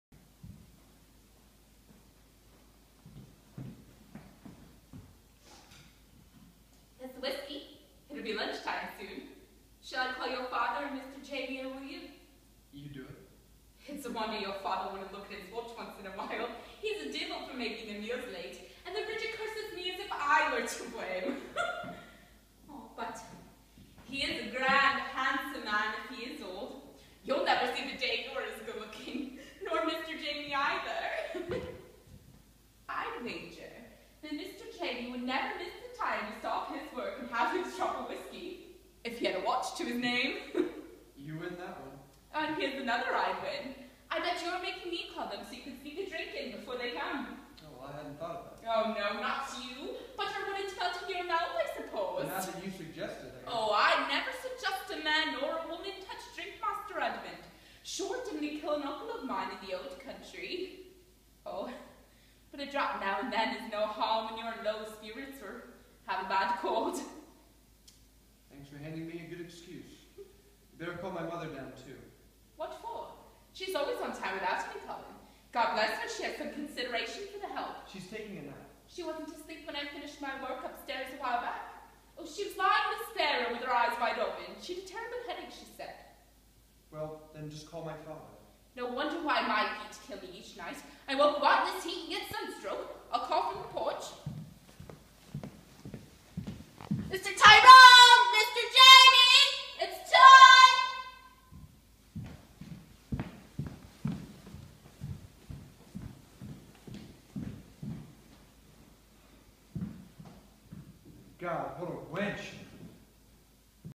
Sneak Preview 3 – “Long Day’s Journey Into Night”
Sam Chapin as Edmund and Melissa Noonan as Cathleen, rehearsing a scene from “Long Day’s Journey Into Night,” Eugene O’Neill’s Pulitzer Prize winning masterpiece. Intimately staged in-the-round at the Eclipse Grange Theater -- you'll feel like you're right in the Tyrone's sitting room!

The show opens Friday, October 10th at 7:00pm and runs for 3 weekends.

Public performances:
Fridays and Saturdays - October 10 & 11; 17 & 18; 24 & 25 at 7:00pm
(Please note earlier start time)

Sunday Matinees - October 19 & 26 at 2:00pm